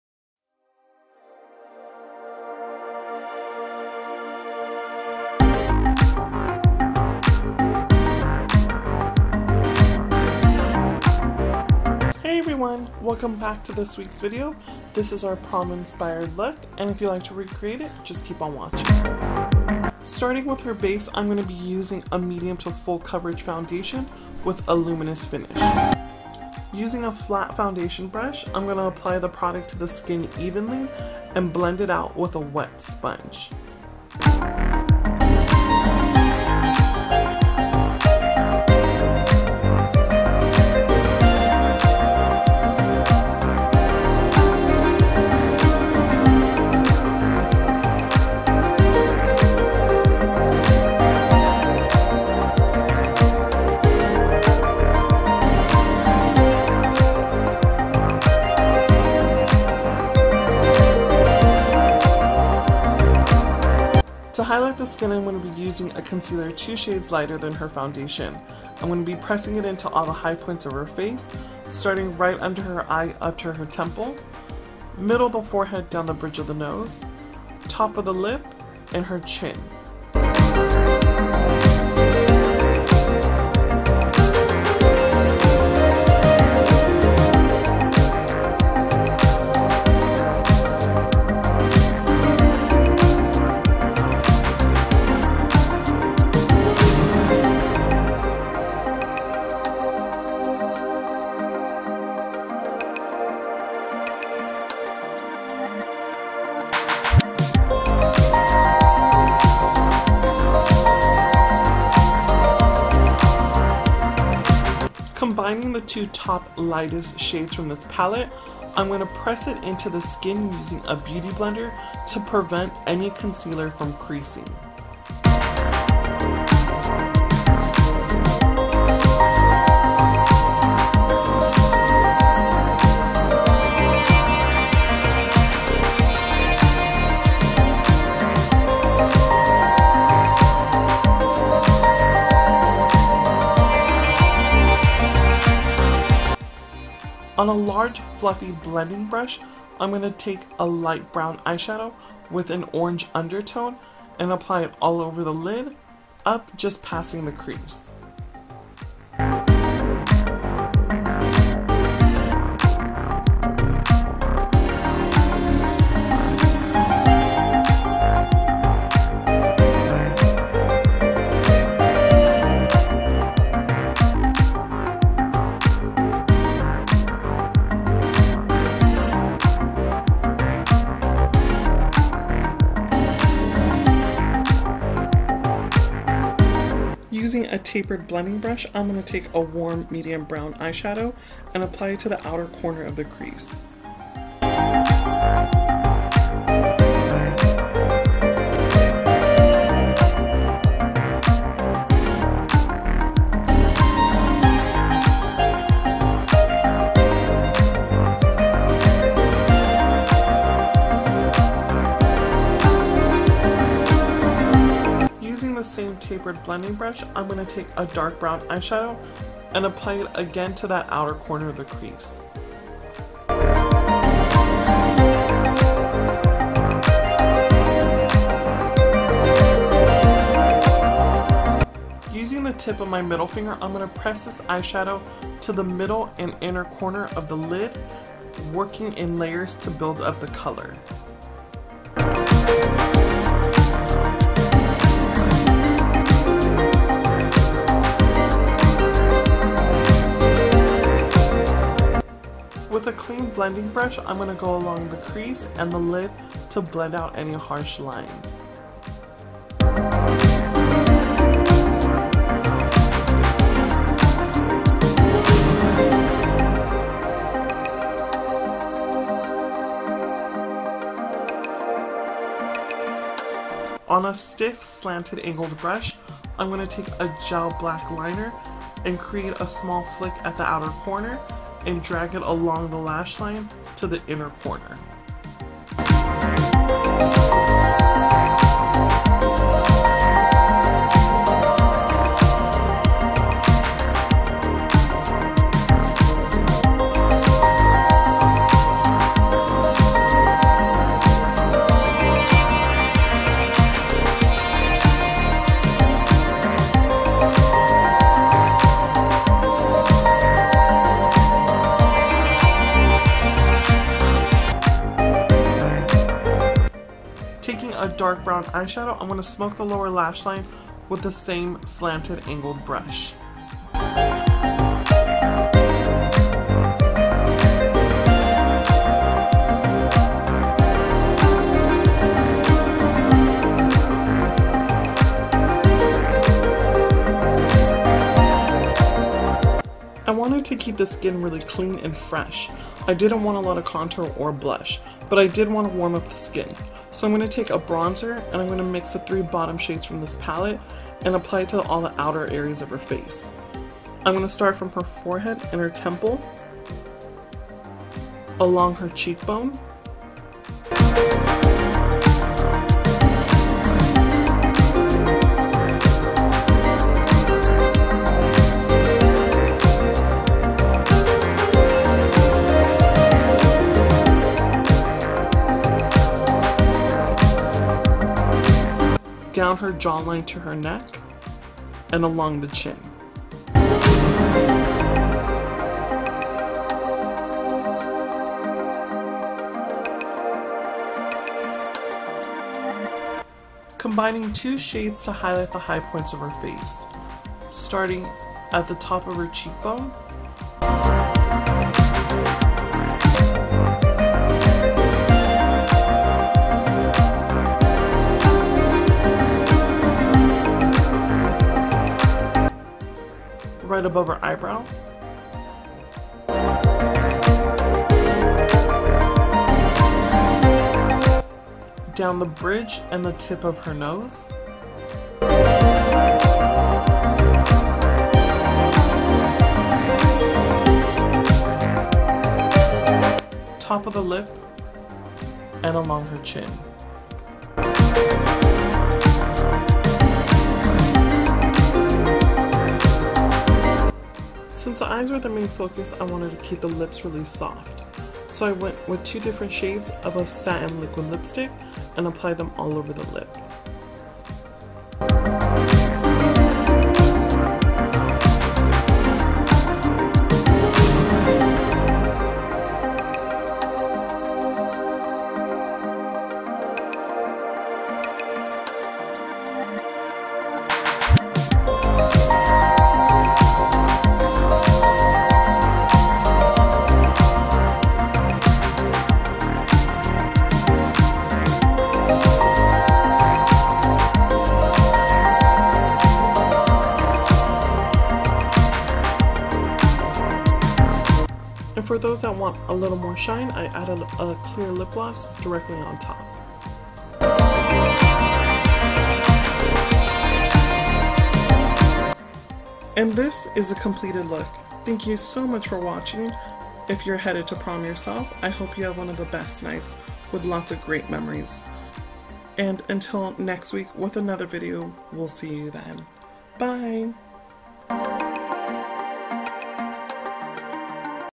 PROM | SPECIAL EVENT MAKEUP
Hey everyone!
Proms are popping up everywhere and we decided to inspire those who are in need of some ideas. I decided to keep the colors nuetral but can be switched up to your preferance. I paired this soft smokey eye with lashes, glowy skin and a peach nude lip. Wishing you have one of the best nights at your prom, dance the night away and make memories of a lifetime!

If you have any ideas, comments or questions please leave them down below!
Xo Claudia for Beauty Blot

PRODUCTS
Loreal True Match Lumi W5
Sonia Kashuk Foundation Brush
Beauty Blender
MAC Cosmetics Pro Longwear Concealer NC15
Lorac Pro Contour Palette
Morphe Brushes Single Shadow - #14
MAC  Cosmetics Eyeshadow - Embark
Makeup Geek - Cocoa Bear
Inglot 7SS Brush
Colourpop Eyeshadow - I Heart This
Sigma E40 Brush
Maybelline Eye Studio Gel Liner - Blackest Black
MAC Cosmetics Powder Blush - Cheek Pollen
Colourpop Highlighter - Wisp | Spoon
Real Techniques Setting Brush
ELF Blush Brush
Colourpop Satin Lip Cream -  Magic Wand | Echo Park
Lancome Juicy Tubes - Spring Fling

OFF CAMERA
Anastasia Beverly Hills Dip Brow - Medium Brown
Everlash Wispies

Sony A5100, 16/50 Lens
Wonder Share Video Editor